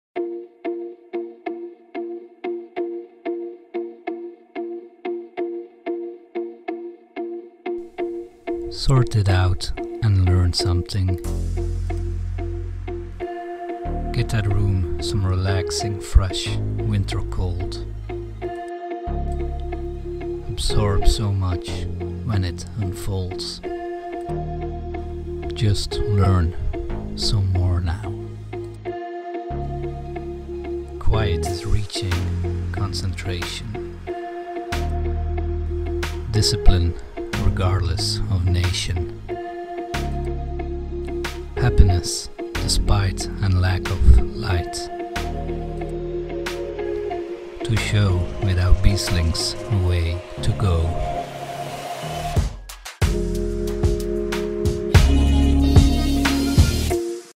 LEARN - Motivational short
Text based on a poem I heard on Internet Archive.
Filmed with DJI Osmo Action
Edited with Vegas Pro
Filmed in summer 2020, somewhere in Belgium.